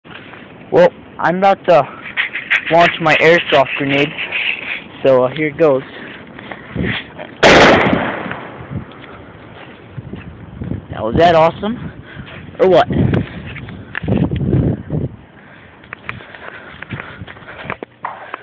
BB grenade launch!!!!! Sounds like real grenade
This is so awsome! it really worked and sounded like a real grenade. ingredients: baking soda, toilet paper, vinegar, empty water bottle, and BBs. so awsome try it.